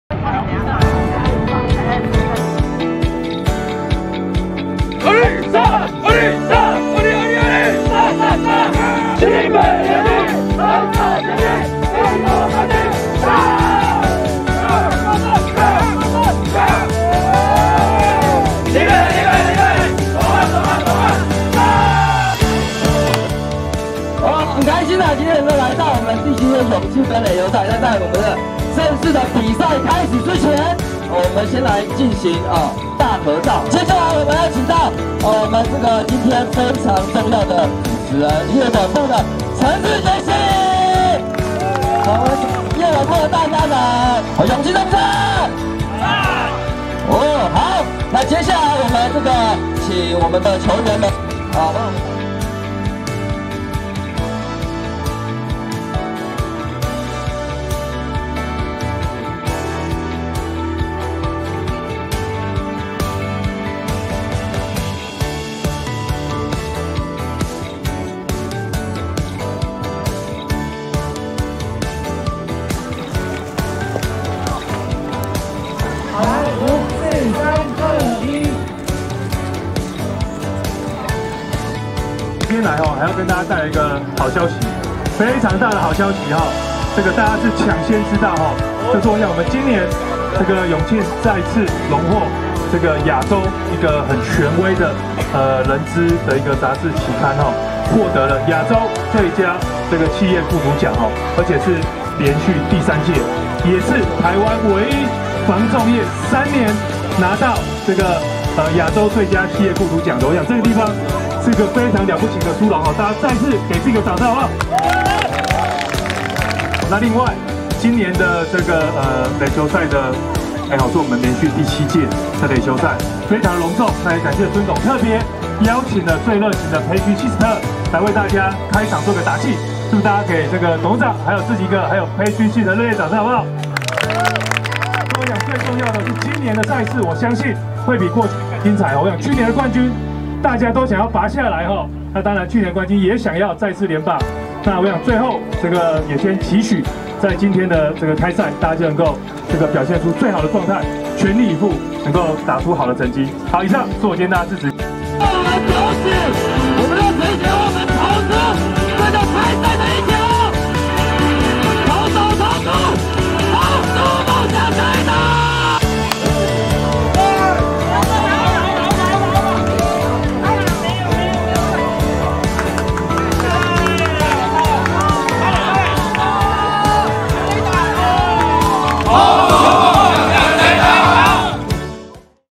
【影片】第七屆永慶盃壘球賽開打 PS女孩開場舞嗨翻全場
房仲第一品牌永慶房屋於今9月9日體育節舉辦「第七屆永慶盃壘球賽」開幕式，現場有超過一百六十位球員、上百位親友組成的加油團齊聚一堂，永慶房屋還特別邀請知名職棒啦啦隊Passion Sisters表演動感十足的開場舞，熱情、活力的舞蹈為現場球員們注入一股熱情與活力，現場氣氛超級嗨。